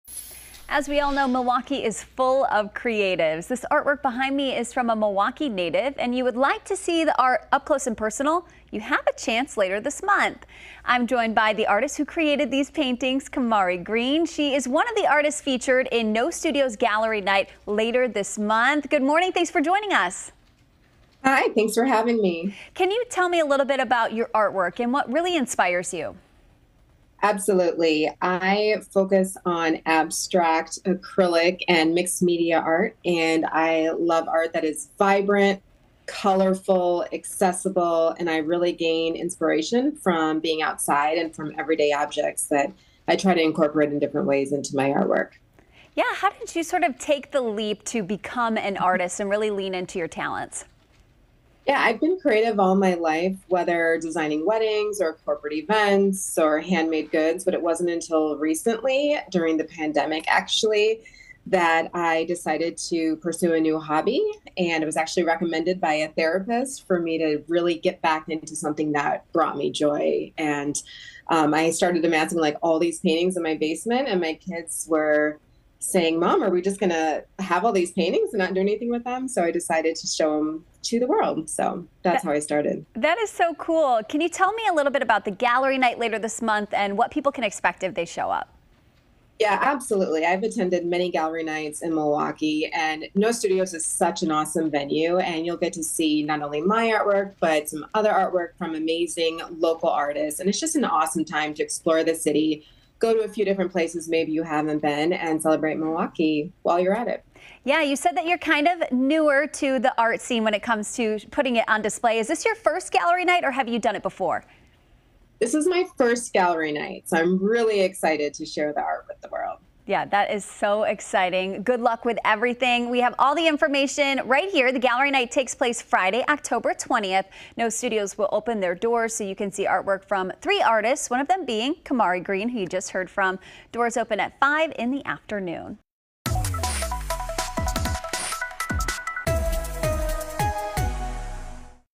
Milwaukee artist has paintings on display at No Studios for Gallery Night
Artist Kamari Green's paintings will be on display at No Studios during Galley Night on Oct. 20.